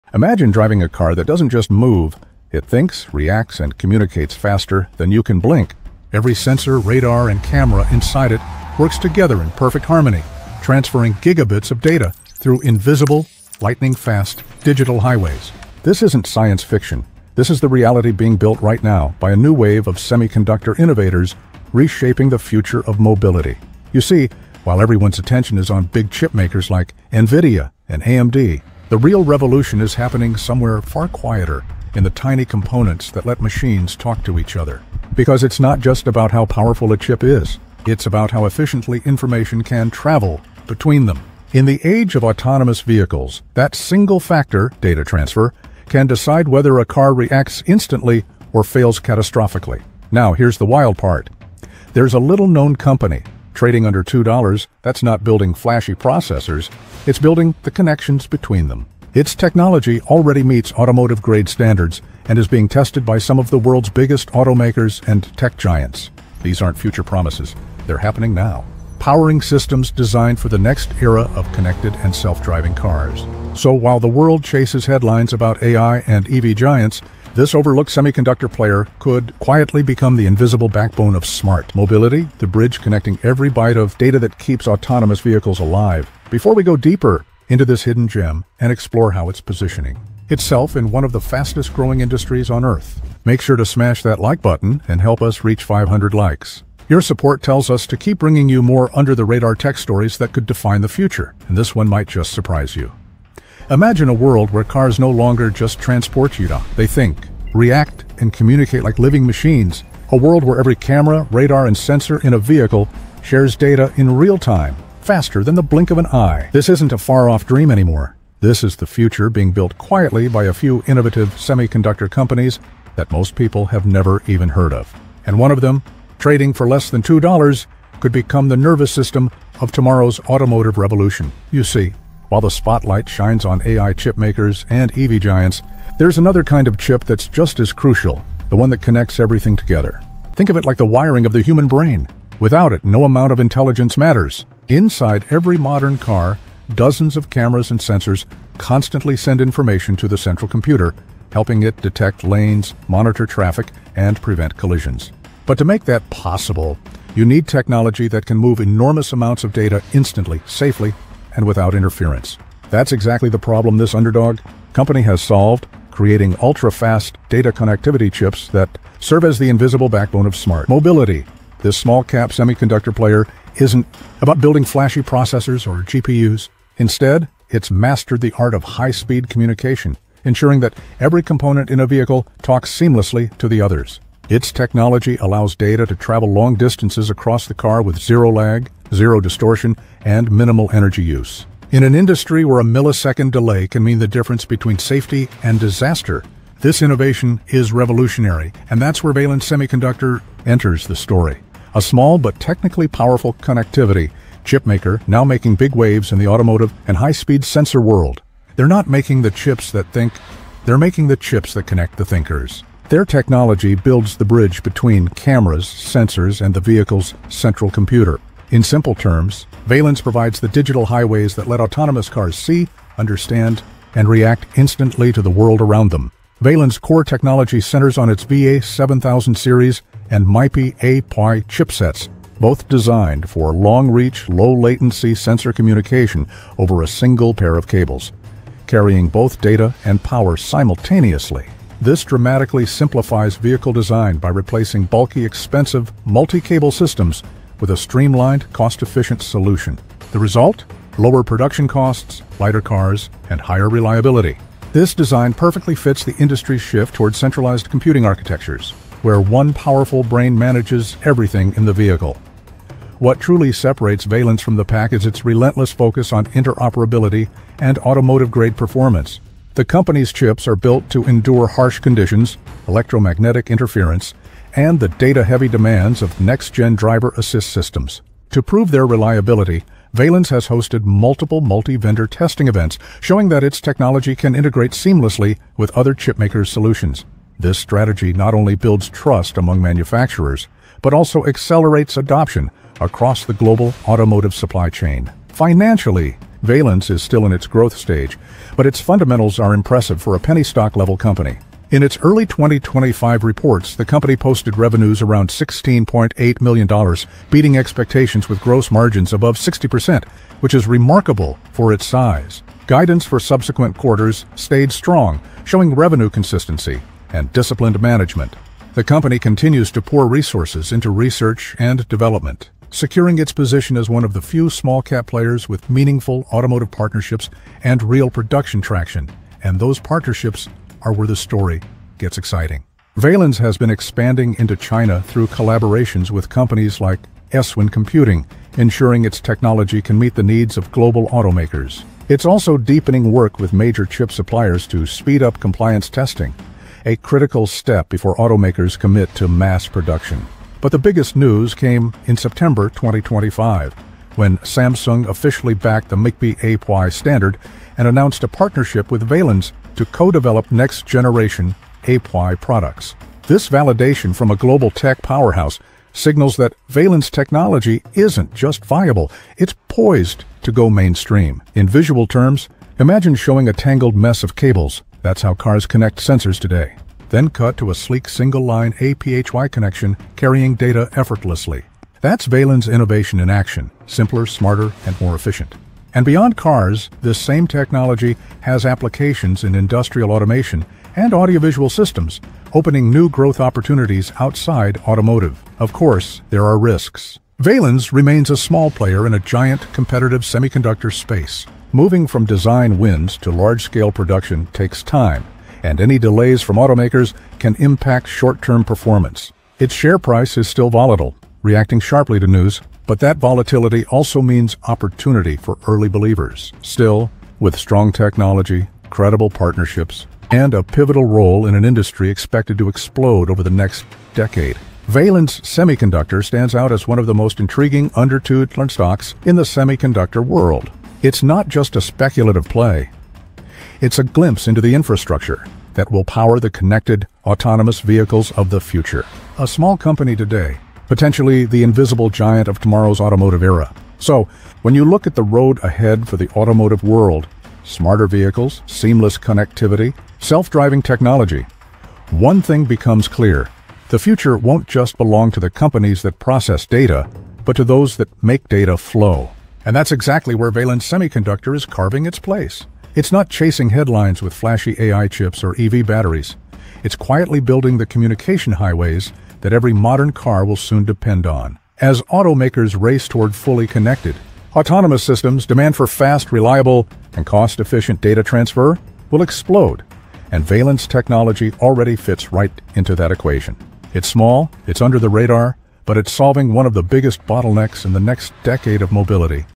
🔥 This $1.67 Penny Stock Might Be the Next Big Automotive Chip Play! 🚀
The semiconductor revolution is quietly reshaping the future of cars, AI, and data connectivity — but not every breakthrough comes from billion-dollar chip giants. In this video, we uncover a small, under-$2 semiconductor company that’s building the invisible digital bridges allowing self-driving vehicles and advanced driver-assist systems (ADAS) to communicate faster, safer, and smarter.

From high-speed data transfer between sensors and processors to cutting-edge automotive-grade reliability, this company’s technology could soon become the nervous system of the next-generation vehicle. It’s working behind the scenes with major global partners to accelerate adoption of ultra-fast communication standards that will define the future of connected and autonomous driving.

We’ll break down how this little-known player’s technology reduces wiring complexity, lowers production costs, and enhances data integrity — all while positioning itself right in the middle of one of the fastest-growing markets in tech. With new partnerships emerging and automotive demand for connectivity chips skyrocketing, this might just be one of the most underrated stories in the semiconductor world today.

Whether you’re a tech enthusiast, long-term investor, or someone who simply loves discovering the next hidden gem before it explodes — this video will give you a deep insight into a company quietly powering the future of smart mobility.